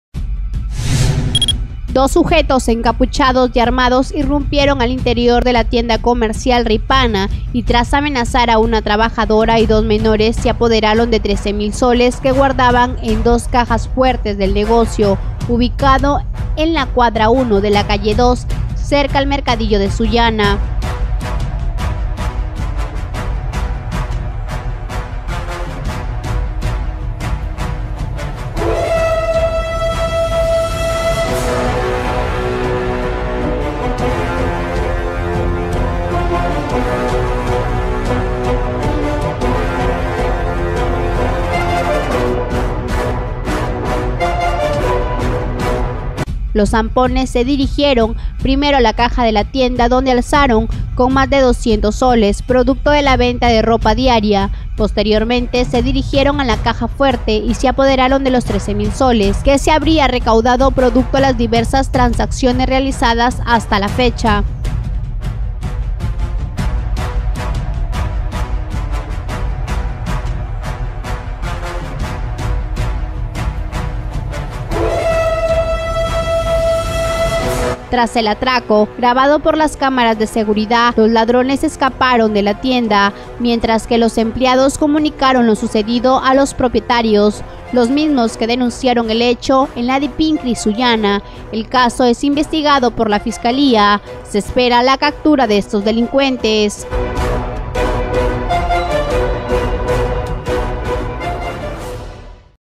Piura: Delincuentes se llevan 13 mil soles de tienda
Delincuentes irrumpieron al interior de la tienda comercial "Rimapa" y tras amenazar a una trabajadora, se apoderaron de 13 mil soles que se guardaban en dos cajas fuertes. Esto sucedió en la provincia de Sullana.